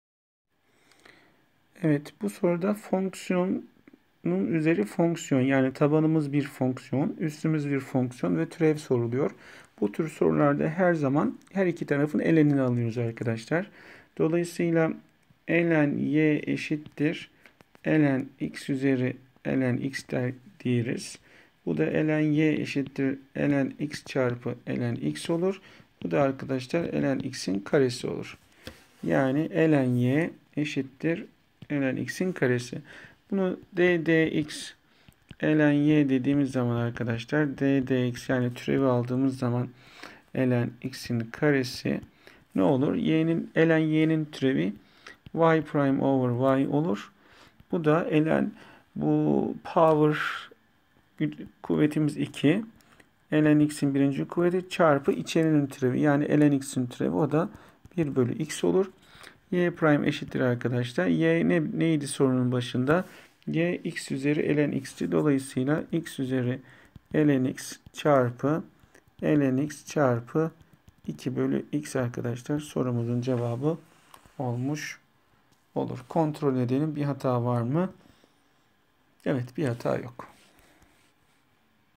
X üzeri ln x in turevi , derivative of x to ln x
How to find derivative of x to ln x , x uzeri ln x in turevi nasil bulunur